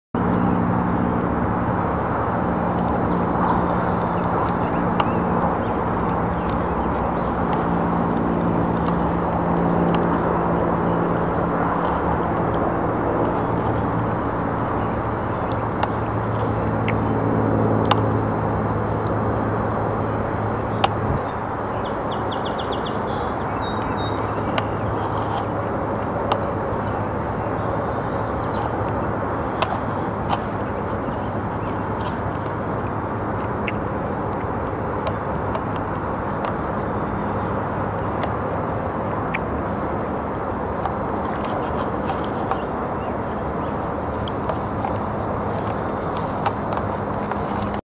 swamp bugs-0004.mp4
Weevil - probably a "clay" weevil - filmed within 1" of subject using Canon HV20 HD cam - 1080p
Location: Whitby, Ontario, Canada